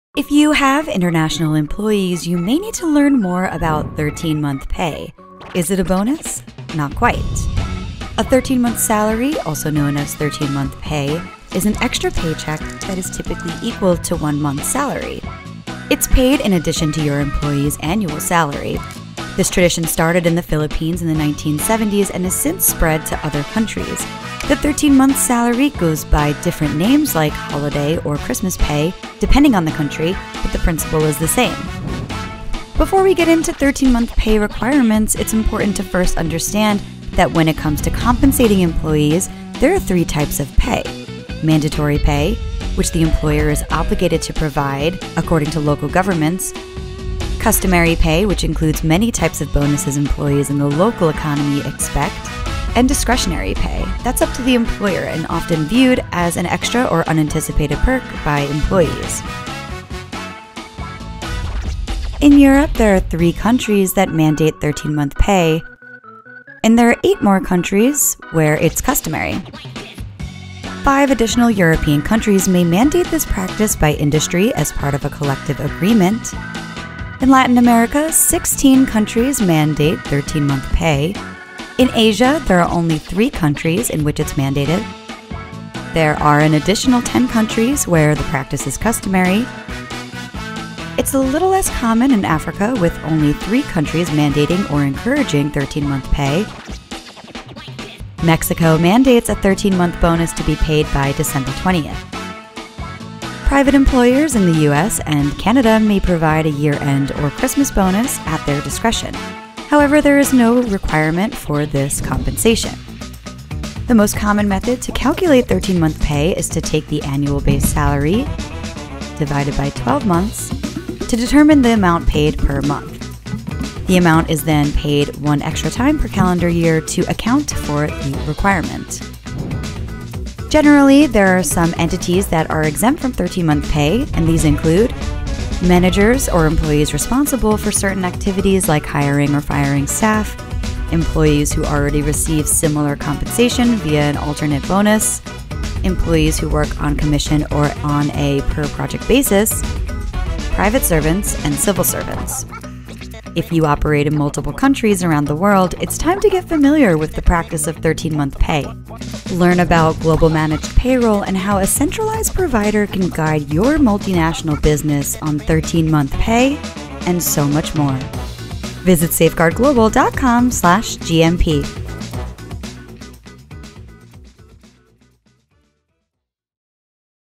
What is 13 month pay?
Did you know that an extra salary payment, often called 13 month pay, is customary—or in some cases required—in many countries around the world? If you’re hiring employees internationally, it’s important to understand this salary tradition to ensure your payroll is both compliant and competitive.

Find out which countries require or suggest a 13 month pay as part of an employee’s compensation